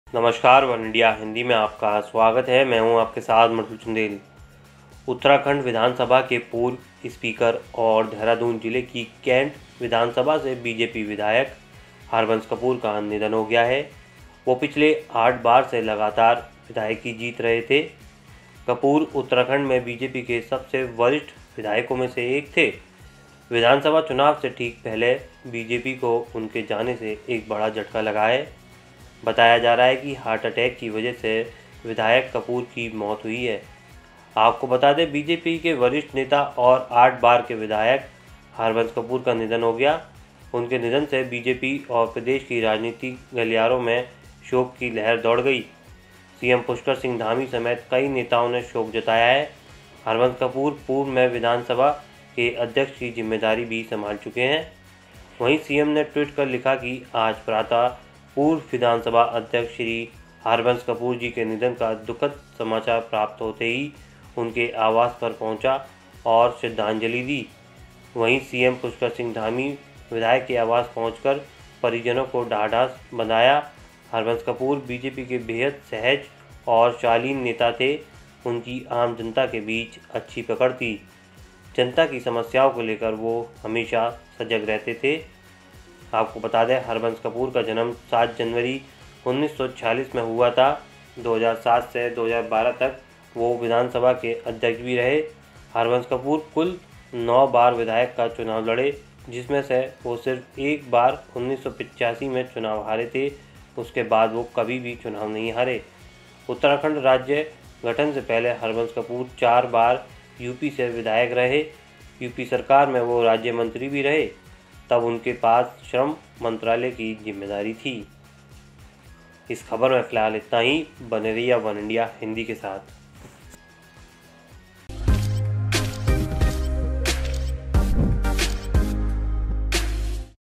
Harbans Kapoor Passes Away: उत्तराखंड के पूर्व स्पीकर और BJP MLA हरबंस कपूर का निधन | वनइंडिया हिंदी
Harbans Kapoor, former speaker of Uttarakhand Legislative Assembly and BJP MLA from Cantt Assembly of Dehradun district, has passed away. He was winning the legislative assembly continuously for the last 8 times. Kapoor was one of the senior most MLAs of BJP in Uttarakhand. Just before the assembly elections, BJP has suffered a major setback due to his departure.

उत्तराखंड विधानसभा के पूर्व स्पीकर और देहरादून जिले की कैंट विधानसभा से बीजेपी विधायक हरबंस कपूर का निधन हो गया है. वो पिछले 8 बार से लगातार विधायकी जीत रहे थे. कपूर उत्तराखंड में बीजेपी के सबसे वरिष्ठ विधायकों में से एक थे। विधानसभा चुनाव से ठीक पहले बीजेपी को उनके जाने से एक बड़ा झटका लगा है